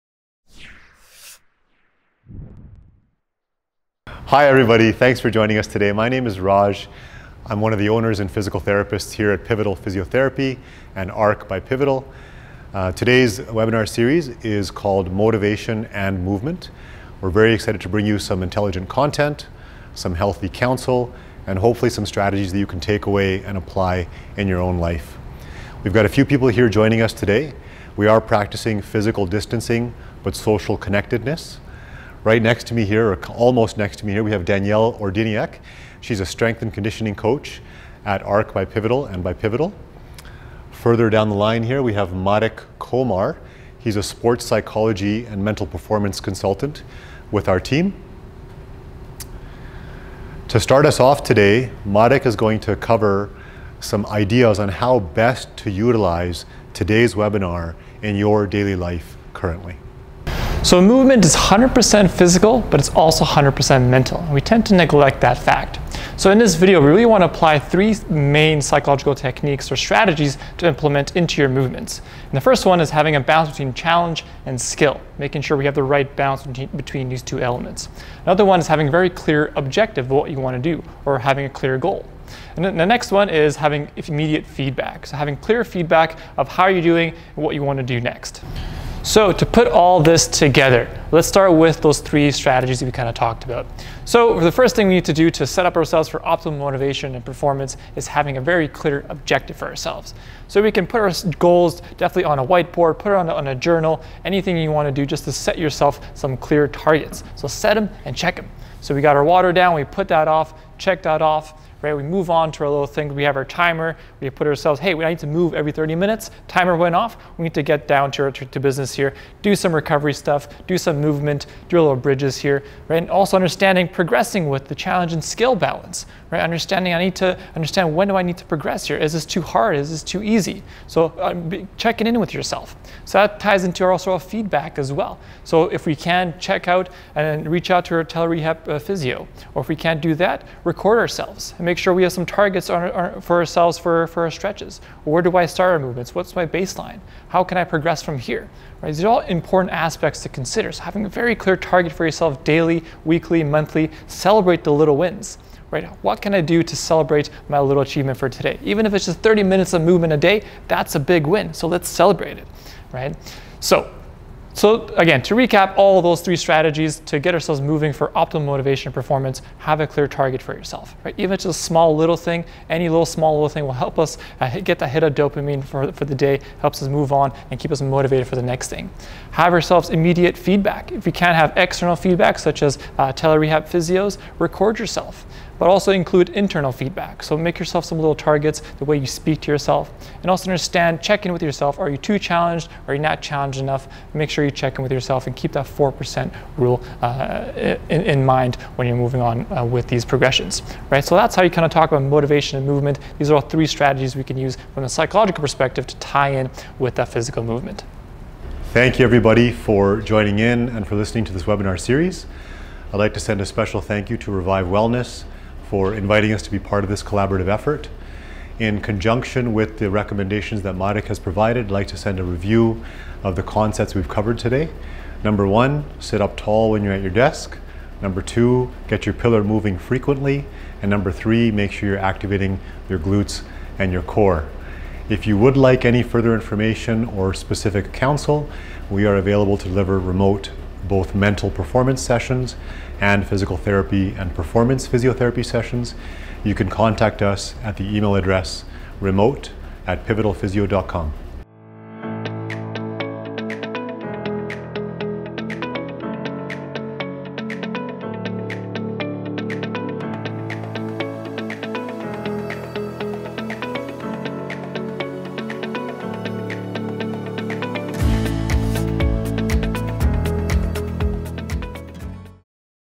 Motivation and Movement Series - Strategic Summary (Video 7)
This is the last video in a series of 7 videos about Motivation and Movement. Stay home, stay safe!

Hosted by Raj, Danielle and Marek from Pivotal Physio.